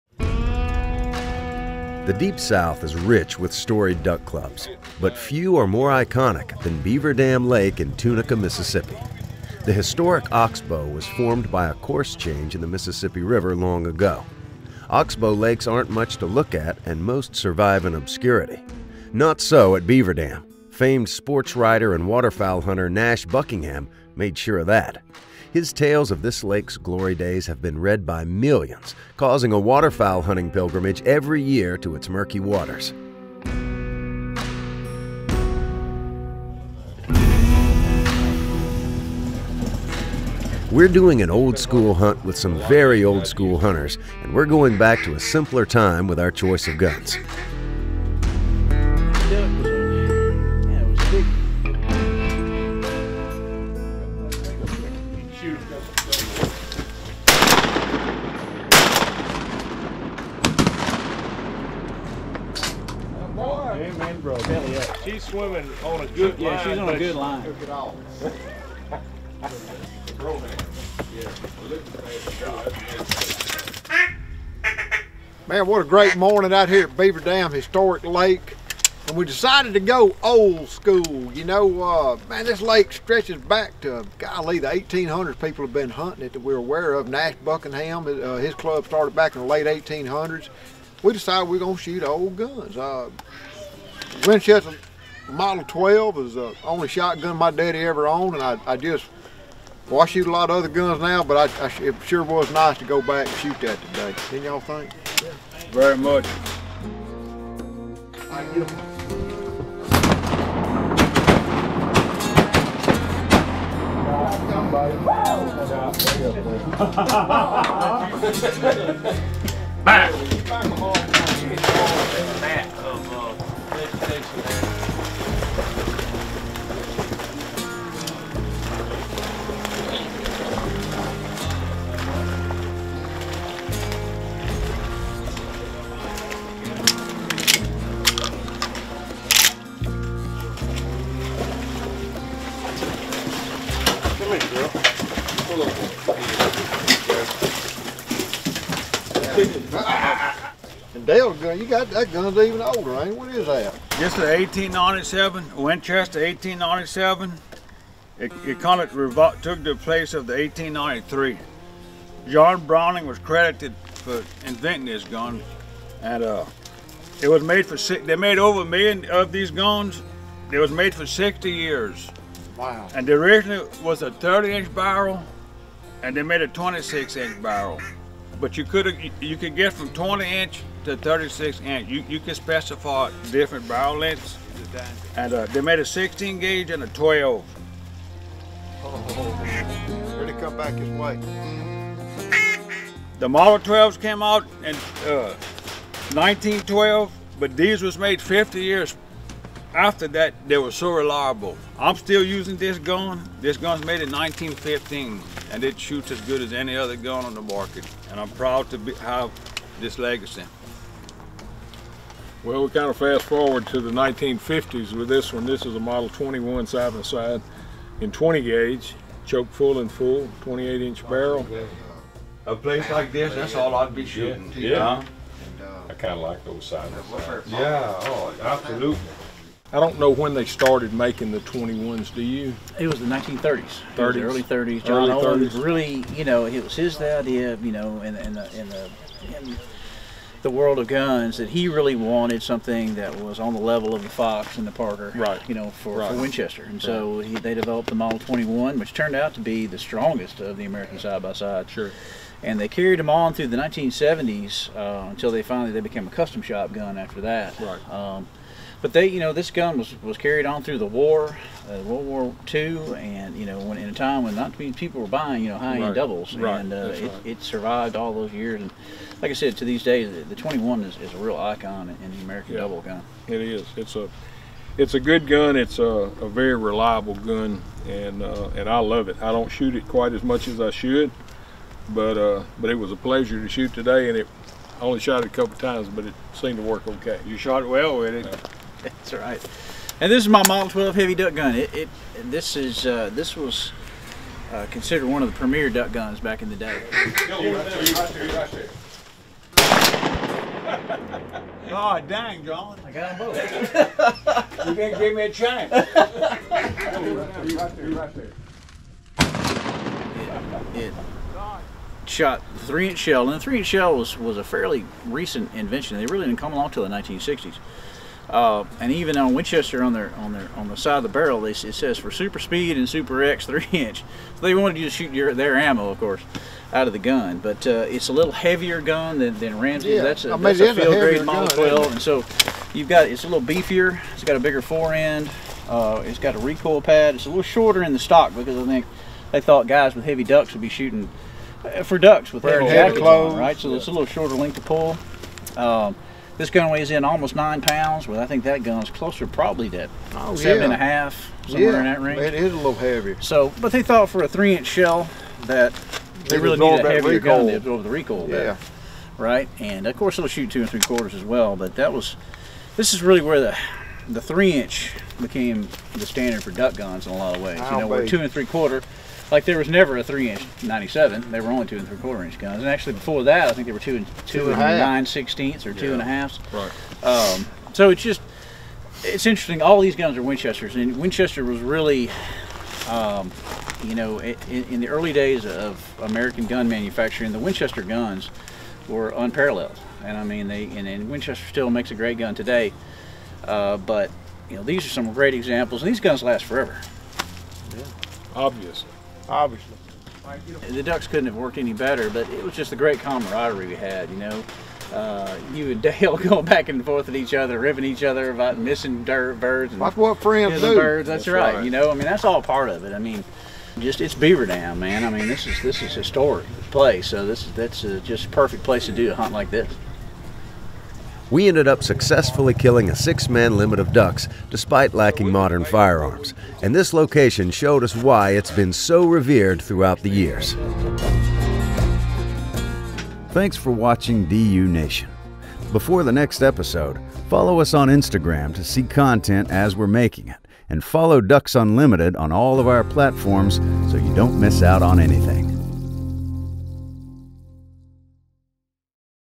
Old School Duck Hunting at Beaver Dam, Mississippi | DU Nation
Old School Duck Hunting at Beaver Dam, Mississippi... on this episode of DU Nation.

Join us as we take a trip back in time using traditional gear and shotguns at this legendary waterfowl hunting spot near Tunica, MS. Bayou Beast Dale Bordelon and Ramsey Russell from GetDucks.com and the Duck Season Somewhere podcast keep traditions alive in old-school waxed cotton while shooting walnut and blue steel. Mike and Lamar Boyd also join the fun on an incredible morning of duck hunting at the location made famous by sporting author and conservationist Nash Buckingham.  The mallards are flying, and the old guns are booming on DU Nation!

Produced by John Gordon
Shot and Edited by Zach Eshleman
Narrated by Jake Phillips